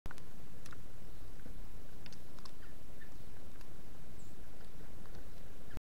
Earl the squirrel is at it again🤣
Earl the squirrel  eating in the snow ❄️ 🤣